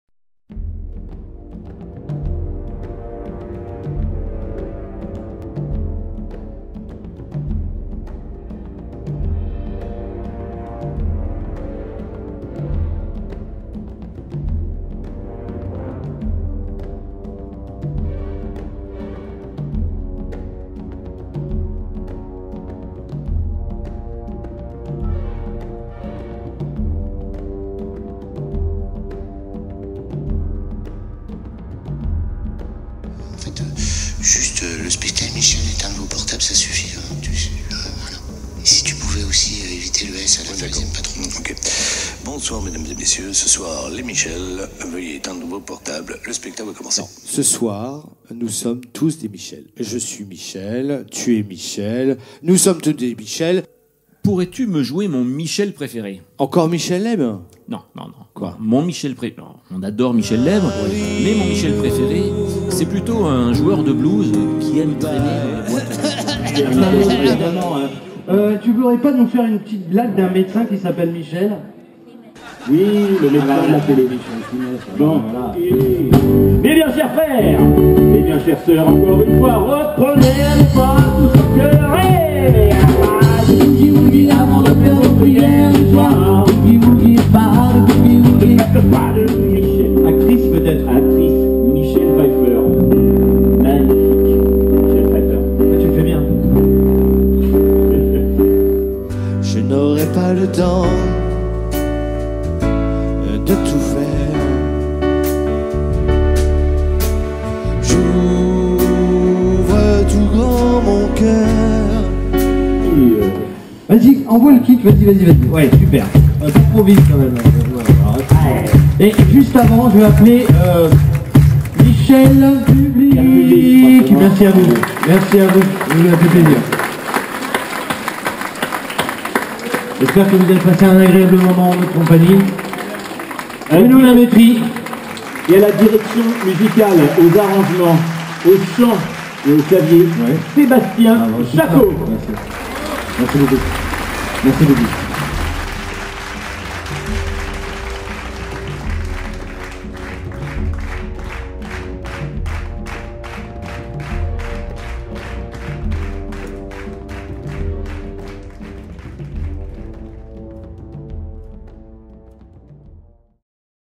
Les Michels en FESTIVAL
Un spectacle live acoustique en festival ou chez vous en privé, à votre bureau en after work, en café théâtre... Une comédie sympa, avec de bonnes musiques !
Piano/chant : Sébastien JACQUOT
Percussions/chant : Bruno LAMETRIE